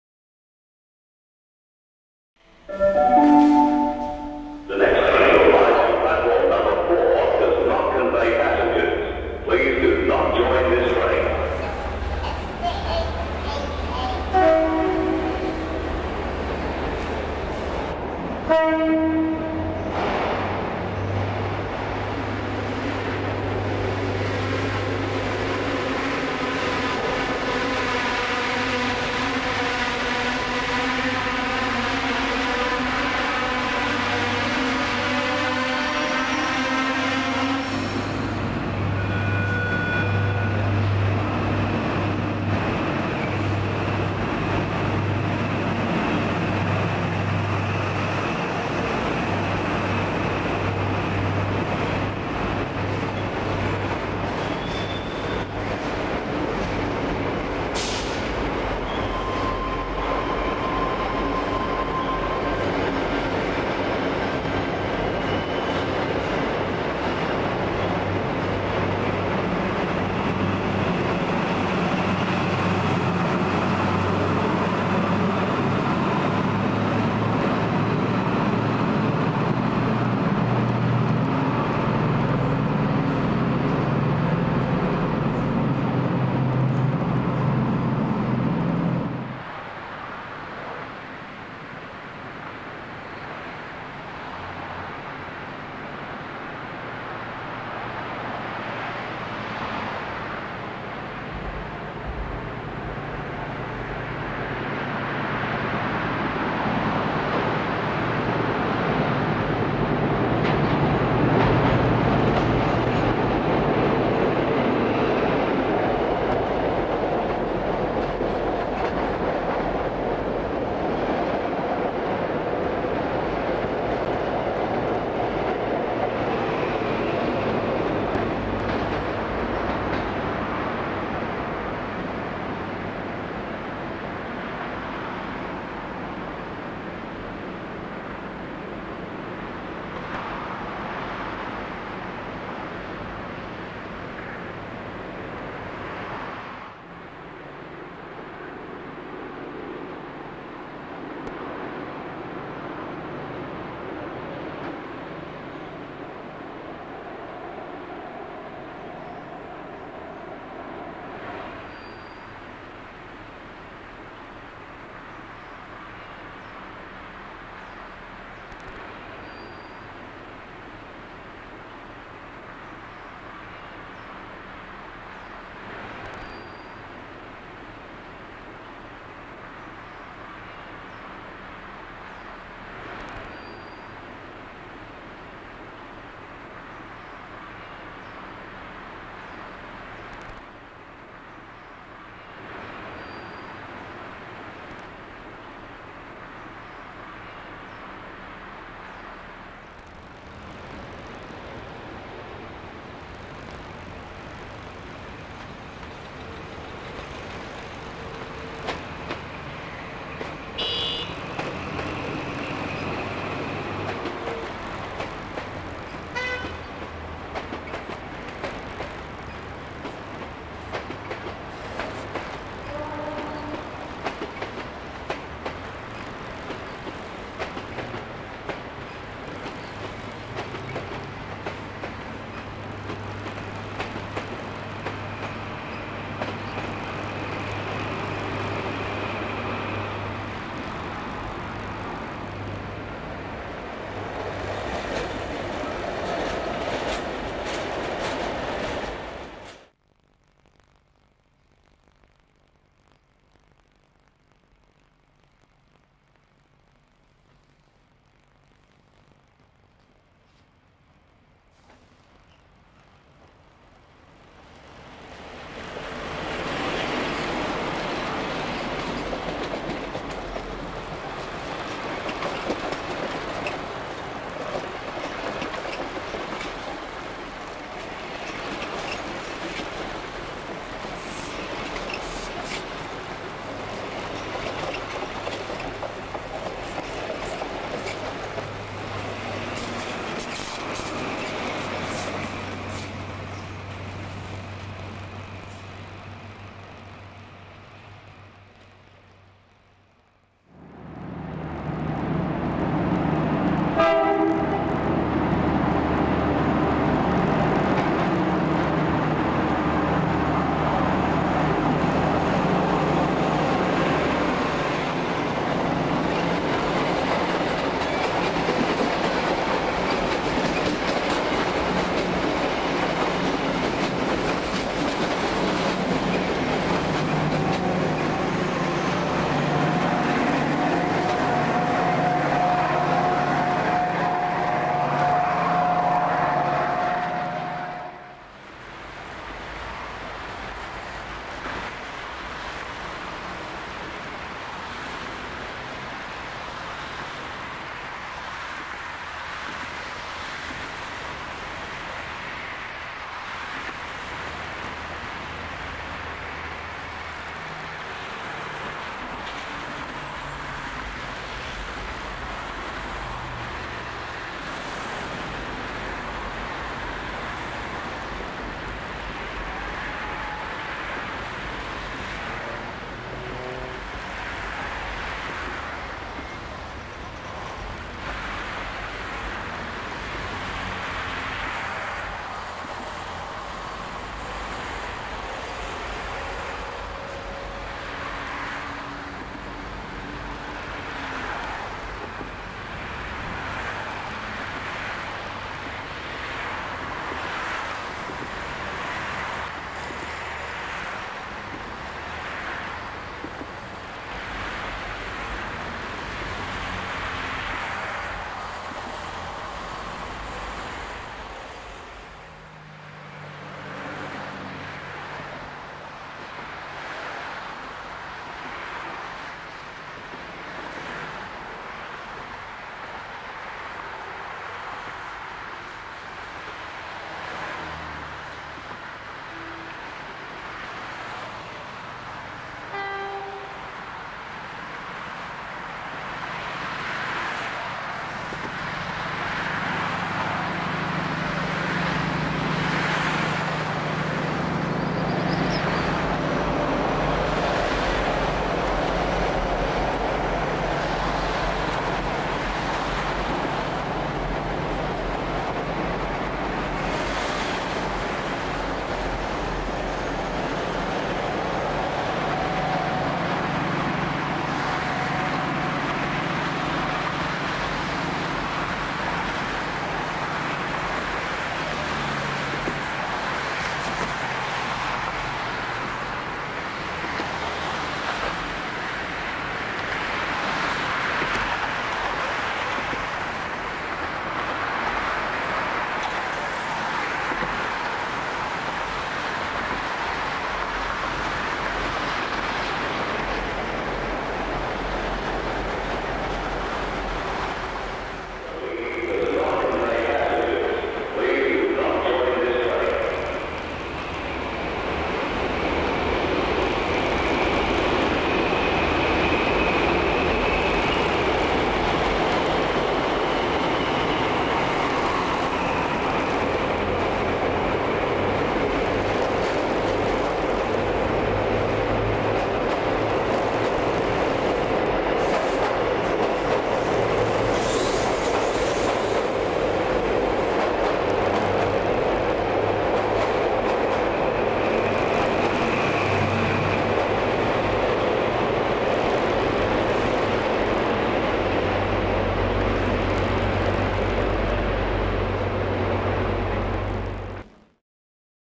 XPT diversions via the North Shore line through Wynyard and over the Harbour Bridge - April 1993
Scenes recorded over Easter in April 1993 of the Brisbane and Murwillumbah XPT services being diverted via the North Shore line due to the closure of the main north line.